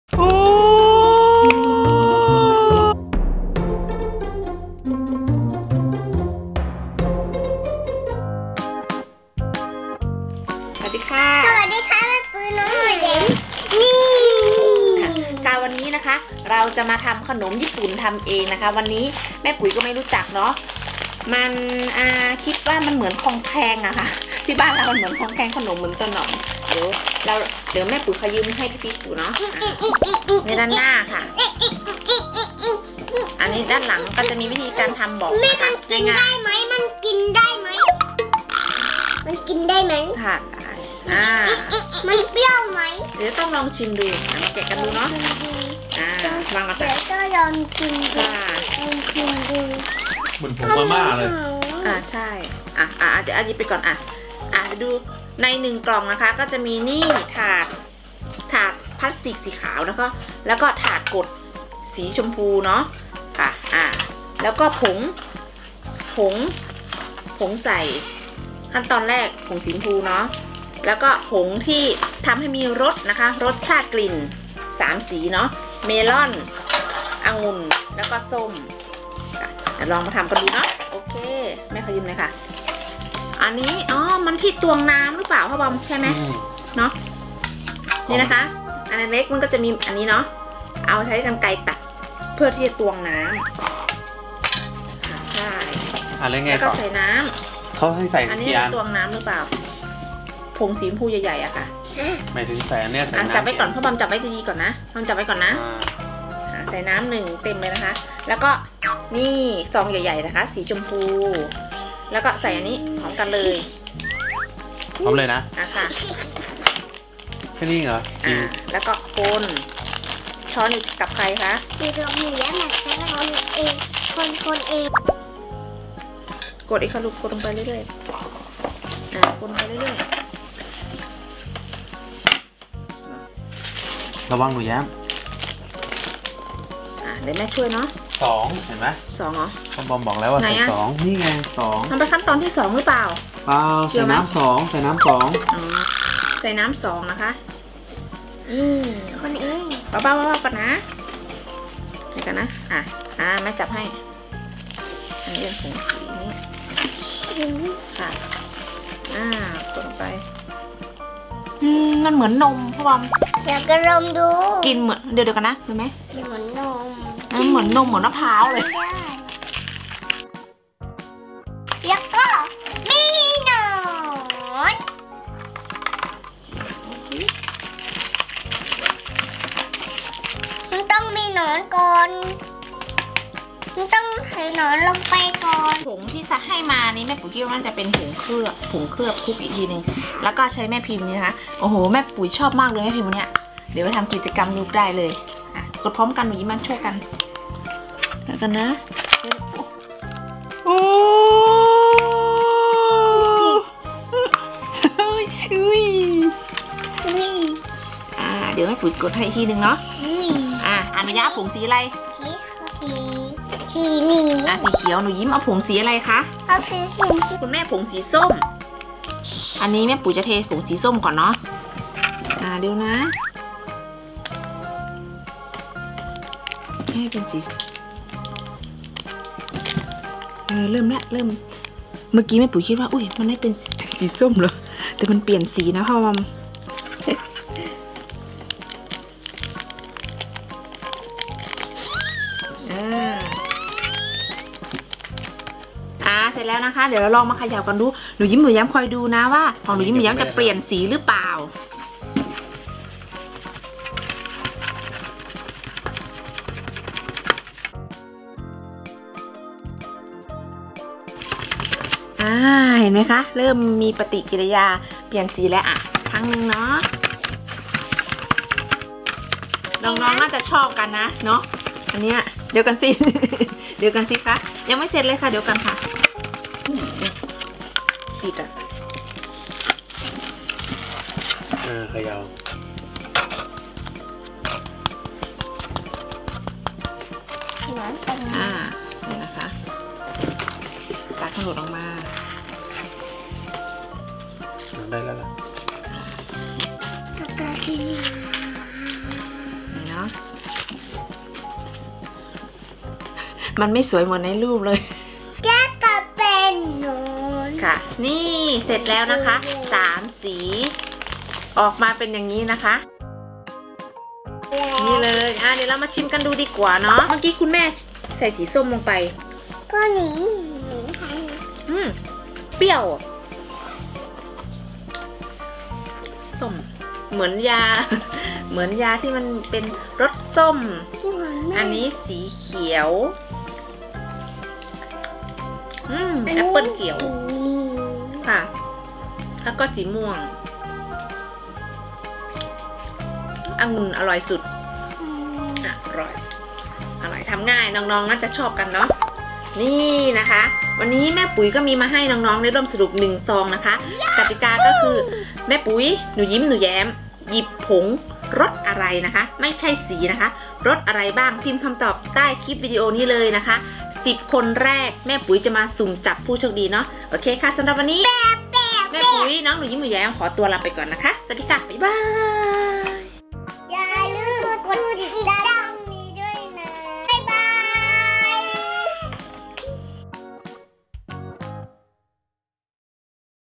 หนูยิ้มหนูแย้ม | รีวิวขนมญี่ปุ่นทำเอง | เหมือนหนอนเลยอึ๋ย!
ฝาแฝด หนูยิ้มและหนูแย้ม คลิปนี้จะมา รีวิวขนมญี่ปุ่นหนอนหลากสี เป็นขนมญี่ปุ่นอีกแล้วอ่านไม่ออกสักตัวเลย แต่ก็มีวิธีทำขนมอยู่ข้างซอง เรามาลองดูว่า มันจะออกมาหน้าตาเป็นยังไงและรสชาติเป็นแบบไหน อะๆอย่าลืมดูด้วยนะท้ายคลิปมีให้ร่วมสนุกกันอีกแล้ว ใครได้ดูต้องตอบได้แน่ๆเลย

ขอบคุณที่รับชมวิดีโอ ถ้าชอบอย่าลืมกด LIKE / SHARE / SUB ให้ด้วยนะคะ
ติดตามผ่านแฟนเพจได้ที่